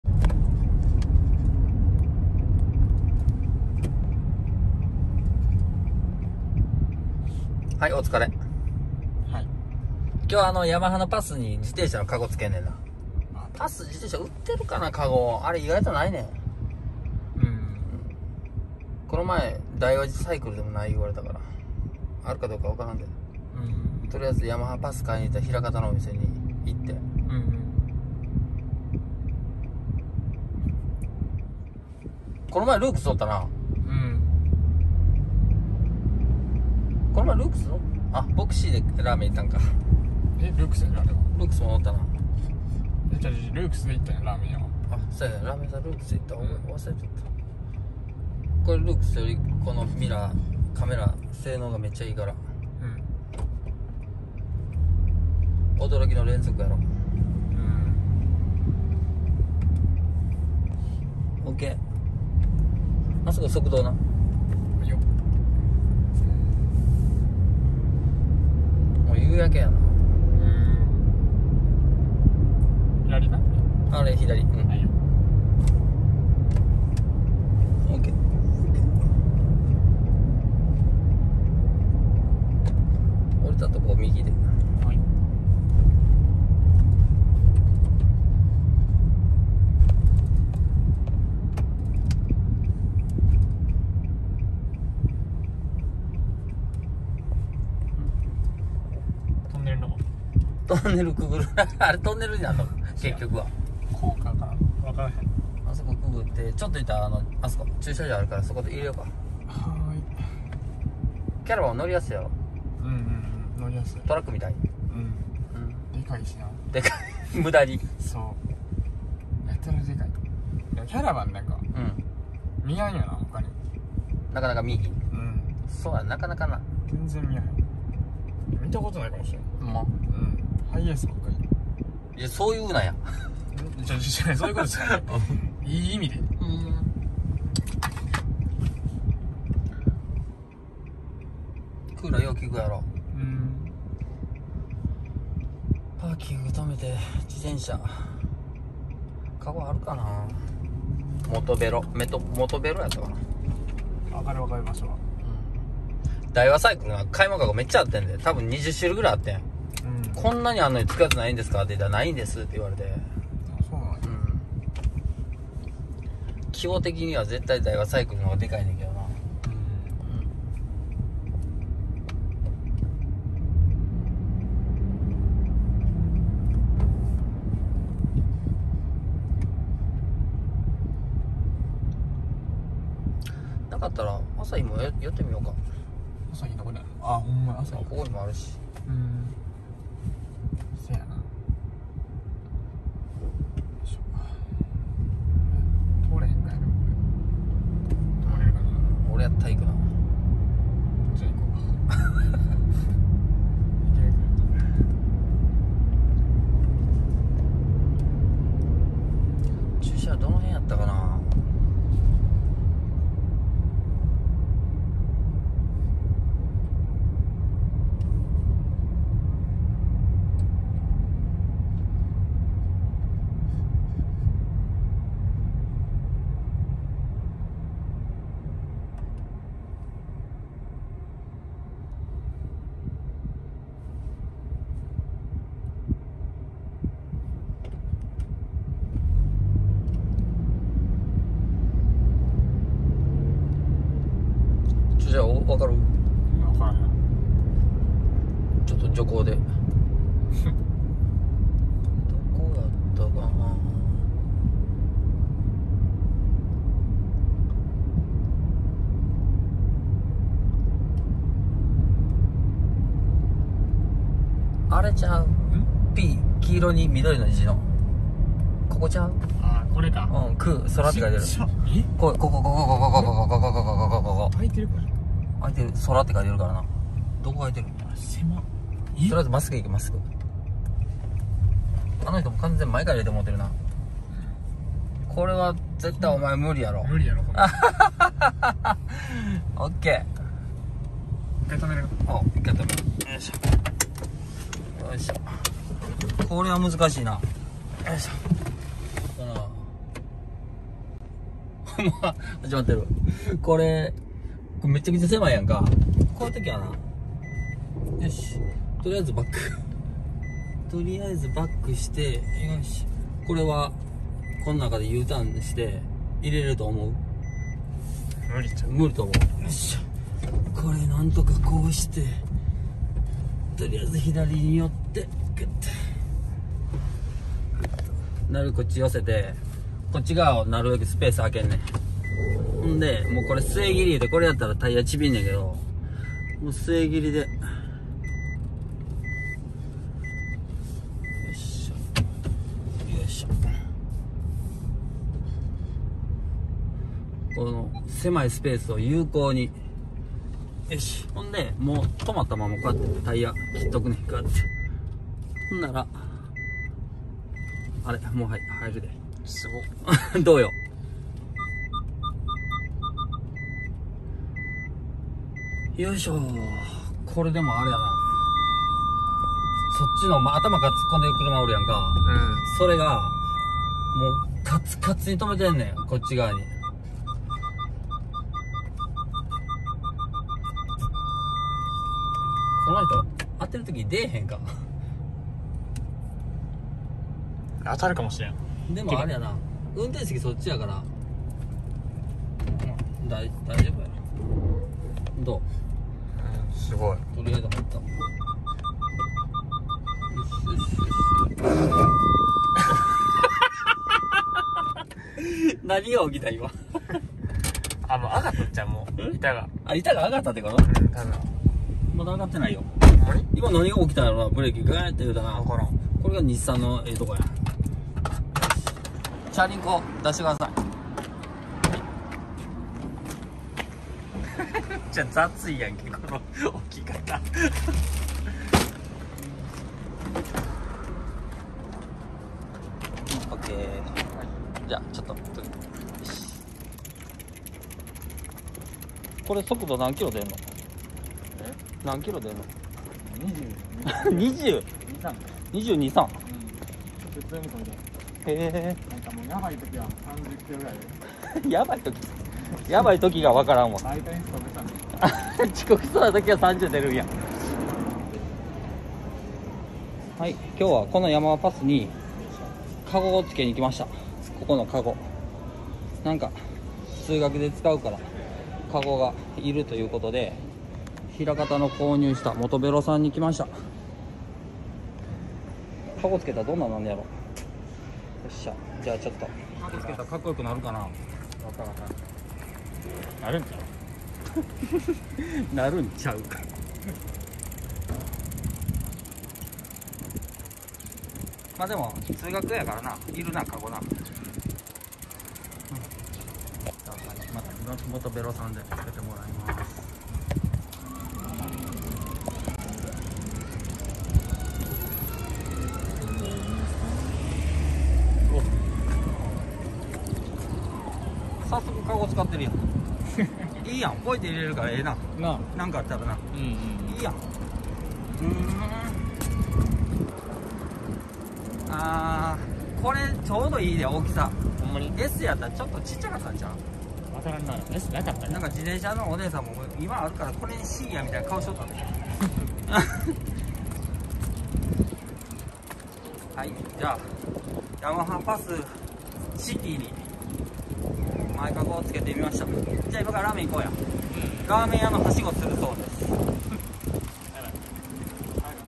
キャラバン チビ運転で電動アシスト自転車YAMAHA PAS-Cに前カゴを取付けに行きました🚗
皆さん　お疲れ様です！こんばんは(^^) 今回は電動アシスト自転車YAMAHA PAS-Cに前カゴを取付けて欲しいと言われたので　購入した枚方市の自転車専門店モトベロ枚方店へ行ってきました💁‍♀️ カゴの取付け工賃込みで7250円でした🚲✨
アルミ製の軽いカゴなのでハンドルも重くならなくて
いい商品でした！
それでは宜しくお願いします
本舗なっか(^^)